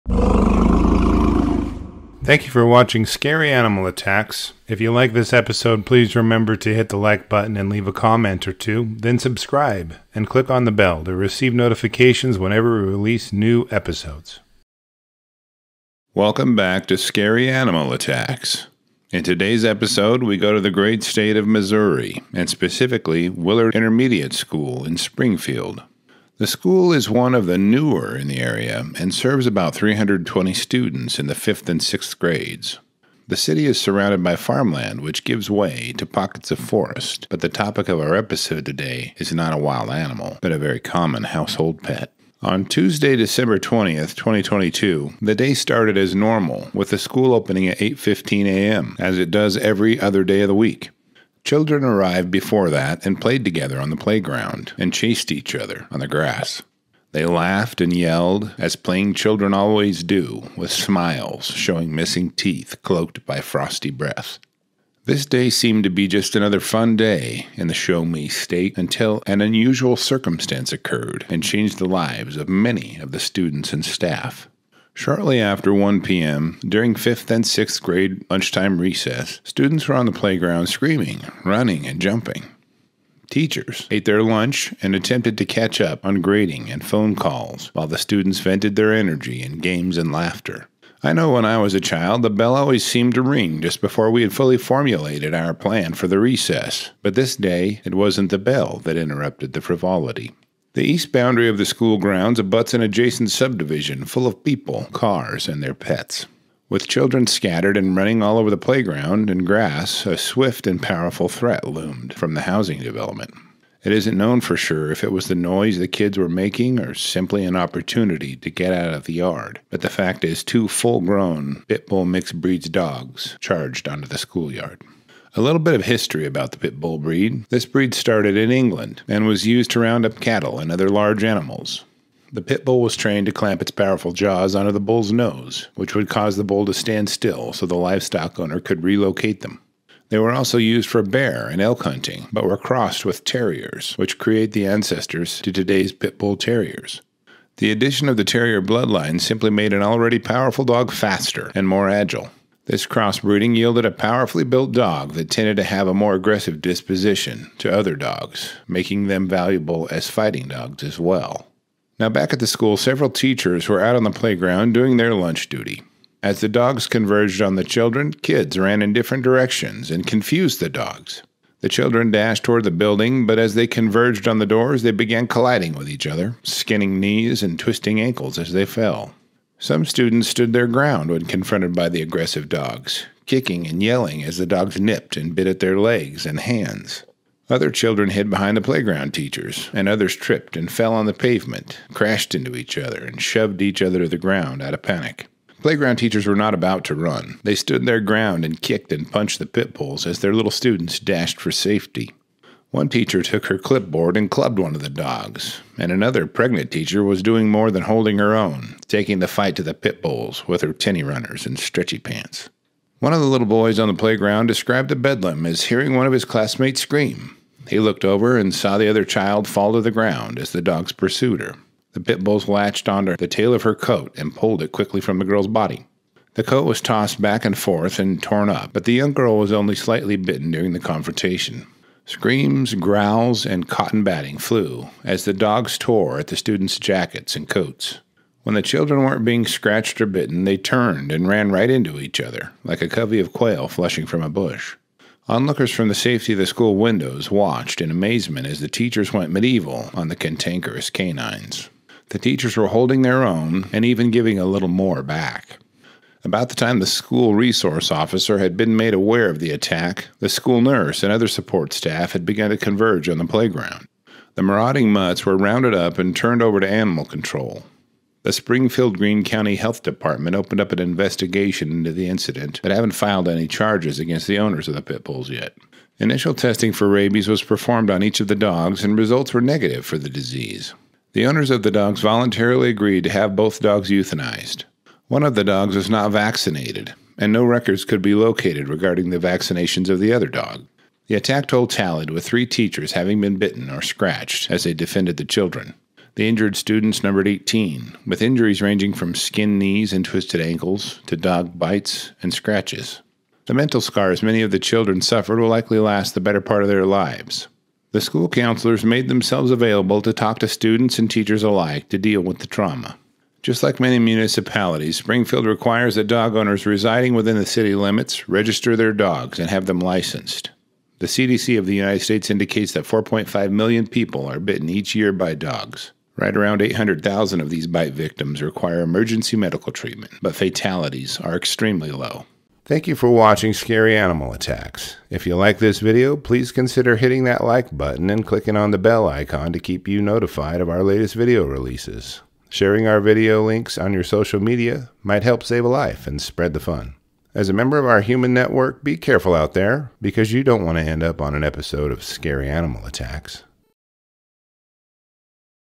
These 2 Pitbulls Attacked A School And Injured 21 People
Welcome to scary animal attacks! In today’s episode, we go to the great state of Missouri, and specifically Willard Intermediate School in Springfield. The school is one of the newer in the area and serves about 320 students in the 5th and 6th grades. The city is surrounded by farmland which gives way to pockets of forest, but the topic of our episode today is not a wild animal, but a very common household pet.
On Tuesday, December 20, 2022, the day started as normal with the school opening at 8:15 AM, as it does on every other day of the week. Children arrived before that and played together on the playground, and chased each other on the grass. They laughed and yelled as playing children always do, with smiles showing missing teeth cloaked by frosty breath. This day seemed to be just another fun day in the Show Me state, until an unusual circumstance occurred and changed the lives of many of the students and staff.
Shortly after 1 PM, during 5th and 6th grade lunchtime recess, students were on the playground screaming, running and jumping. Teachers ate their lunch and attempted to catch up on grading and phone calls while the students vented their energy in games and laughter. I know when I was a child, the bell always seemed to ring just as we had fully formulated our plan for the recess, but this day, it wasn’t the bell that interrupted the frivolity.
The east boundary of the school grounds abuts an adjacent subdivision full of people, cars and their pets. With children scattered and running all over the playground and grass, a swift and powerful threat loomed from the housing development. It isn’t known for sure if it was the noise the kids were making or simply an opportunity to get out of the yard, but the fact is two full grown pitbull mix breed dogs charged onto the schoolyard.
A little bit of history about the pitbull breed. This breed started in England and was used to round up cattle and other large animals. The pitbull was trained to clamp its powerful jaws onto the bulls nose which would cause the bull to stand still so the livestock owner could relocate them. They were also used for bear and elk hunting, but were crossed with terriers which create the ancestors to today's pitbull terriers. The addition of the terrier bloodlines simply made an already powerful dog faster and more agile. This cross breeding yielded a powerfully built dog that tended to have a more aggressive disposition to other dogs, making them valuable as fighting dogs.
Now, several teachers were out on the playground doing their lunch duty. As the dogs converged on the children, kids ran in different directions and confused the dogs. The children dashed toward the building, but as they converged on the doors they began colliding with each other, skinning knees and twisting ankles as they fell.
Some students stood their ground when confronted by the aggressive dogs, kicking and yelling as the dogs nipped and bit at their legs and hands. Other children hid behind the playground teachers, and others tripped and fell on the pavement, crashed into each other and shoved each other to the ground out of panic.
Playground teachers were not about to run. They stood their ground and kicked and punched the pitbulls as their little students dashed for safety. One teacher took her clipboard and clubbed one of the dogs and another pregnant teacher was doing more than holding her own, taking the fight to the pitbulls with her tenny runners and stretchie pants.
One of the little boys on the playground described the bedlam as hearing one of his classmates scream. He looked over and saw the other child fall to the ground as the dogs pursued her. The pitbulls latched onto the tail of her coat and pulled it quickly from the girl's body. The coat was tossed back and forth and torn up, but the young girl was only slightly bitten during the confrontation. Screams, growls and cotton batting flew as the dogs tore at the students' jackets and coats. When the children weren’t being scratched or bitten, they turned and ran right into one another like a covey of quail flushing from a bush.
Onlookers from the safety of the school windows watched in amazement as the teachers went medieval on cantankerous canines. The teachers were holding their own and giving a little more back.
About the time the School Resource Officer had been made aware of the attack, the school nurse and other support staff had begun to converge on the playground. The marauding mutts were rounded up and turned over to animal control. The Springfield-Green County Health Department opened up an investigation into the incident but haven’t filed any charges against the owners of the pitbulls.
Initial testing for rabies was performed on each of the dogs and results were negative for the disease. The owners of the dogs voluntarily agreed to have both dogs euthanized. One of the dogs was not vaccinated and no records could be located regarding the vaccinations of the other dog.